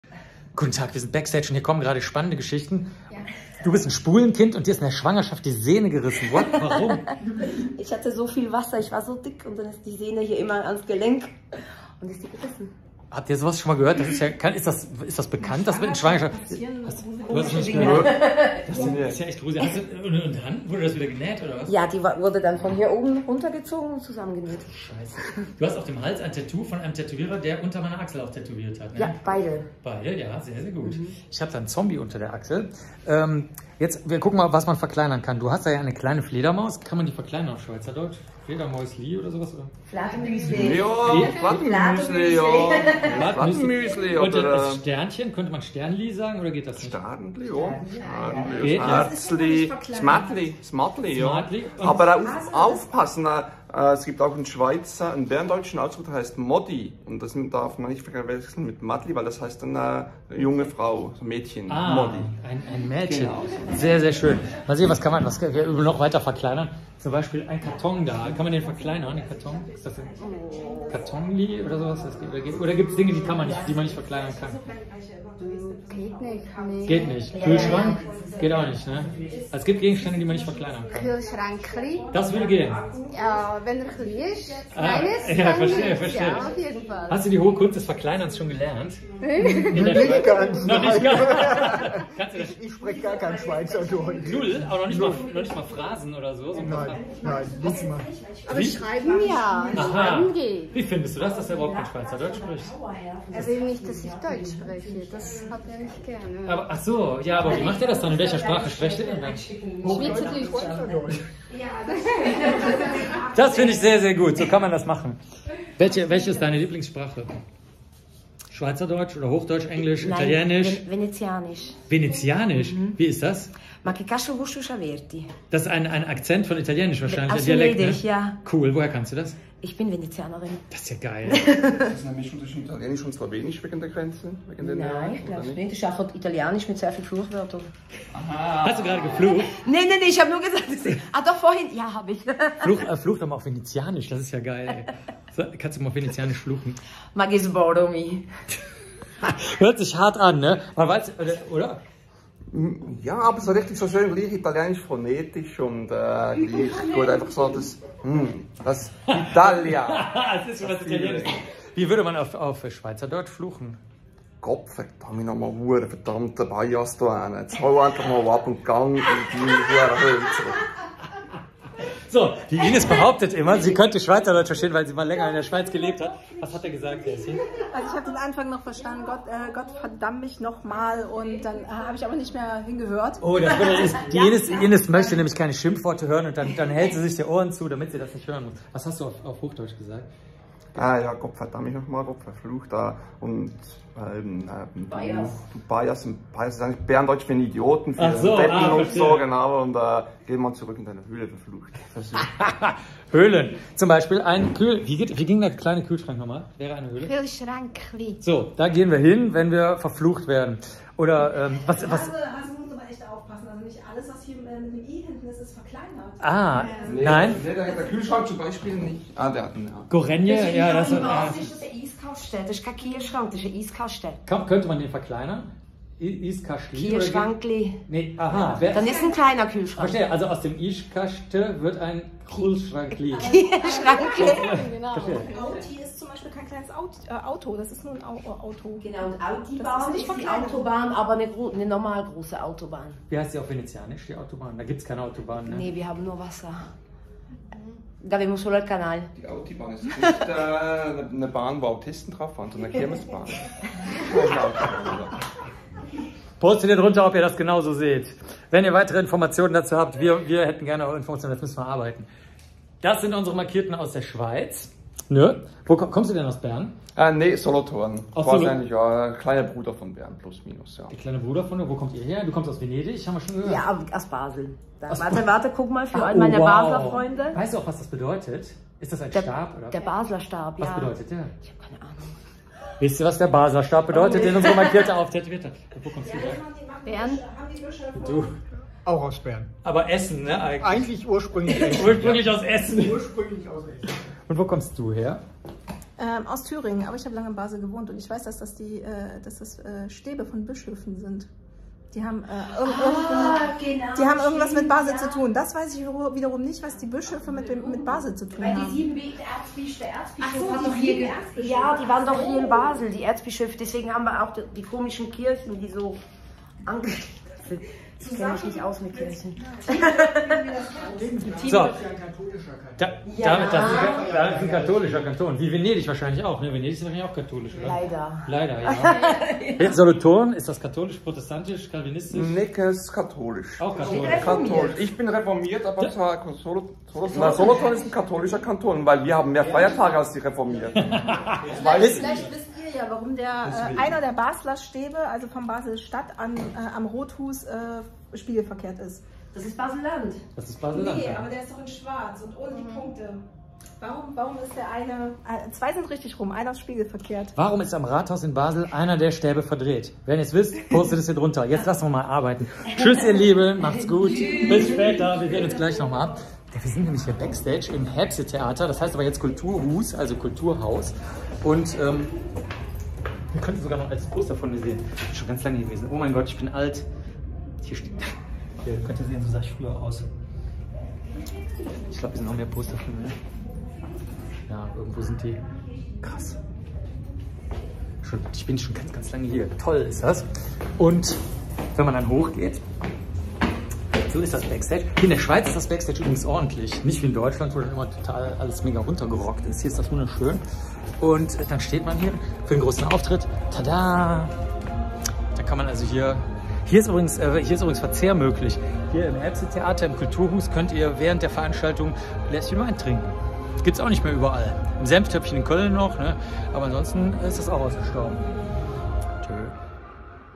Die MARKierten aus Basel 🎭
Live aus dem Kulturhuus Häbse 👌🏻 Die neuesten MARKierten mit Fragen zu gerissenen Sehnen durch Schwangerschaft, dem Basler Stab und Verkleinerungs-Formen auf Schweizerdeutsch 🇨🇭